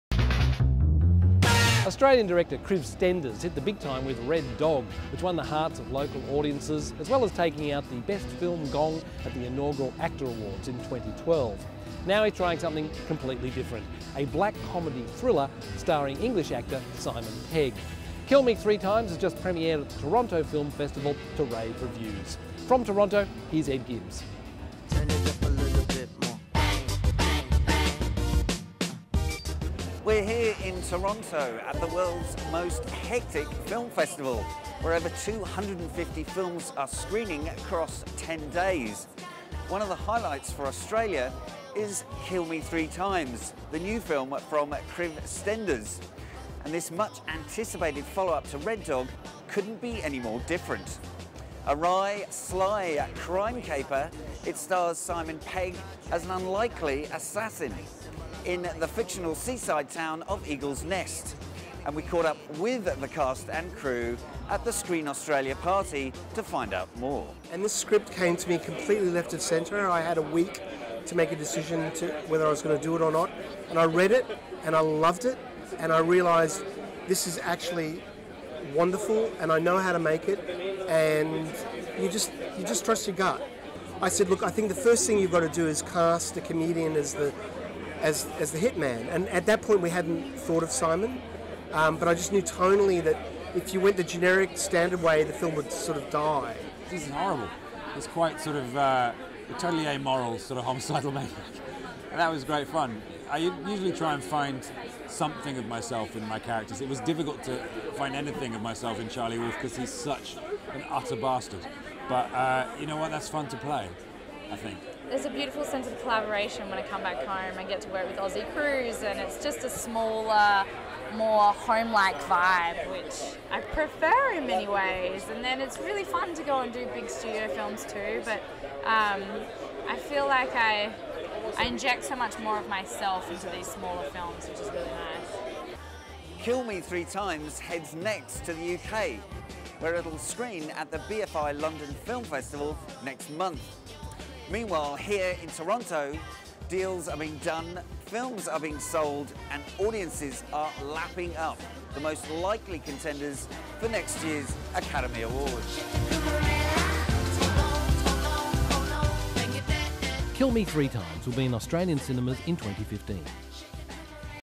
ABC News: Toronto 2014 – Kriv Stenders, Simon Pegg and Teresa Palmer talk KILL ME THREE TIMES
Ed Gibbs reports from the 2014 Toronto International Film Festival (ABC News).